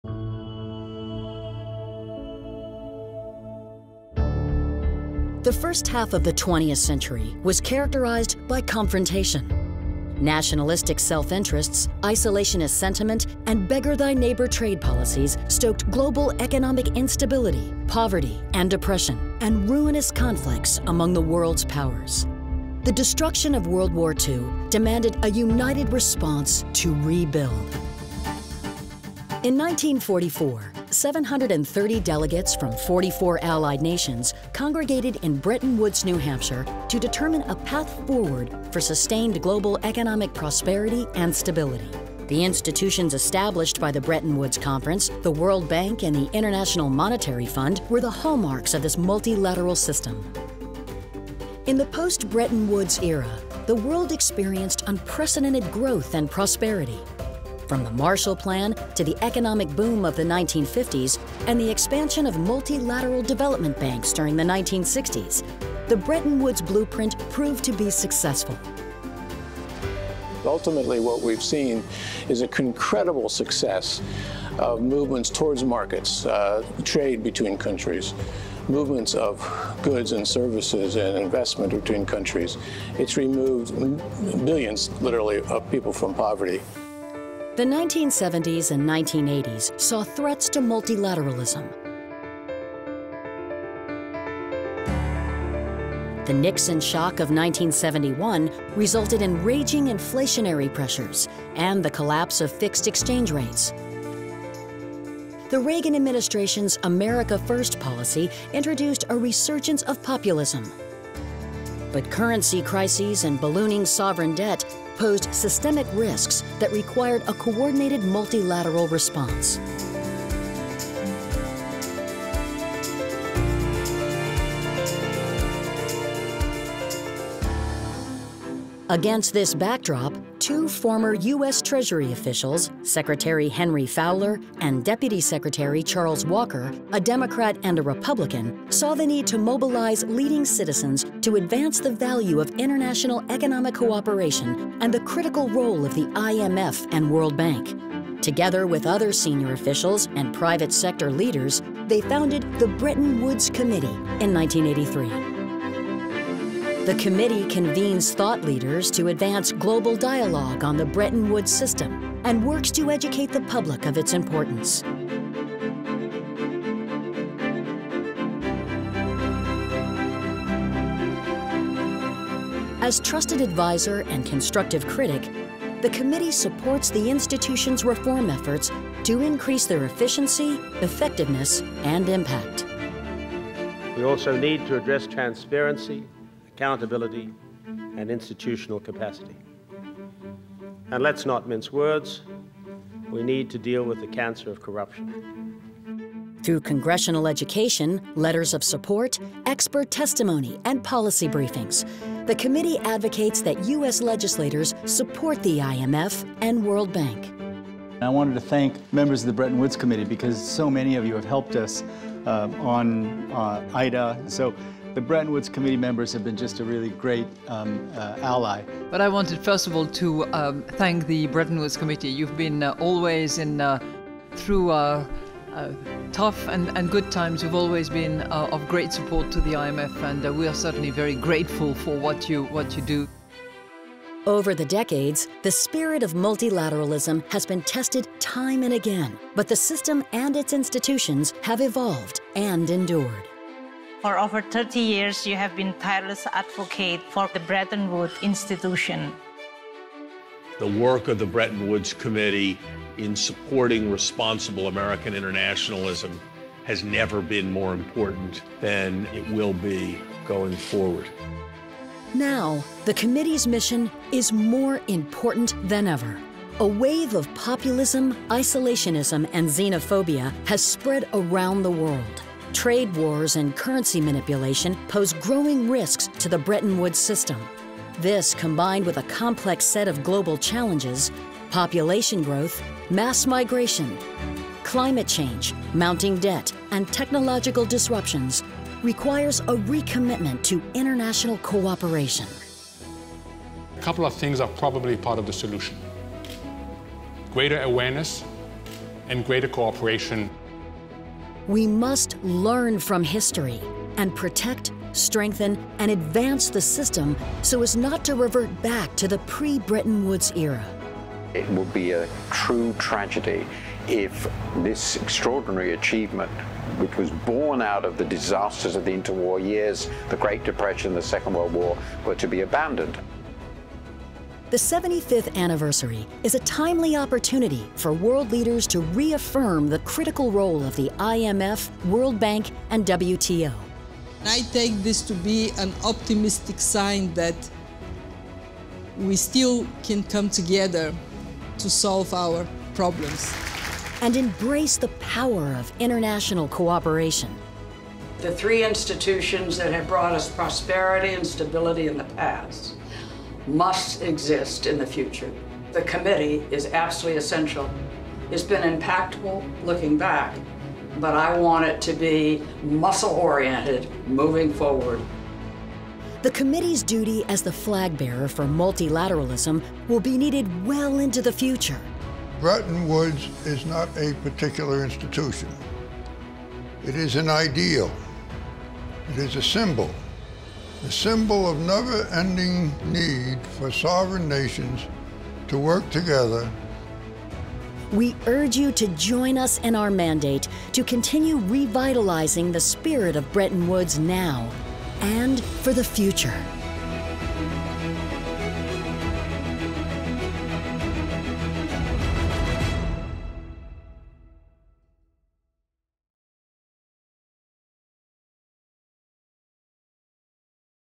About the Bretton Woods Committee
Check out the story of the Bretton Woods Conference and the Committee that was founded to protect its mission.